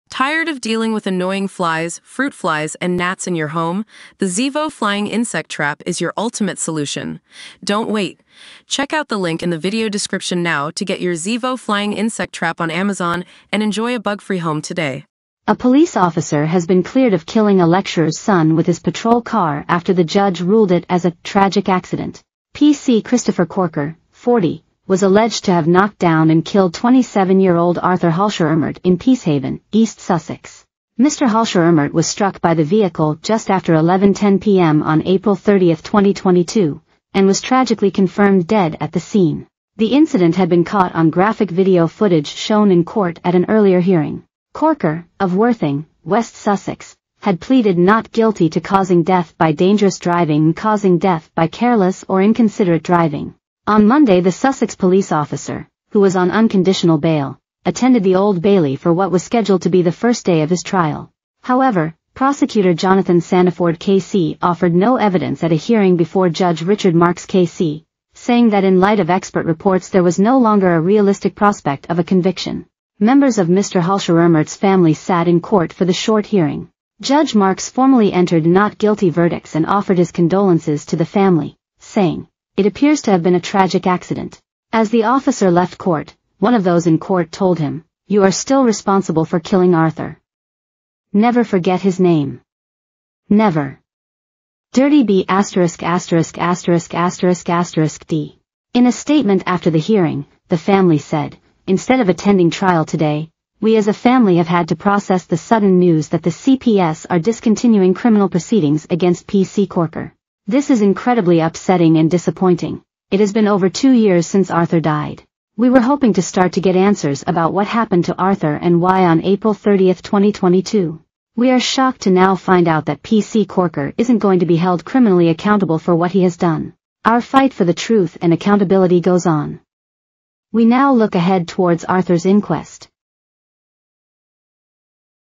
A police officer has been acquitted of killing a teacher's 27-year-old son in an unmarked patrol
A police officer has been acquitted of killing a teacher's 27-year-old son in an unmarked patrol car during a chase, after a judge ruled it was "a tragic accident."